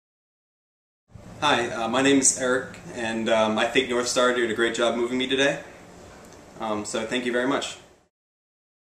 NorthStar Moving - Video Testimonial - Eric - 07.18.2009
Eric moved Locally with NorthStar Moving from Redondo Beach, CA to Huntington Beach, CA. Check us out on the web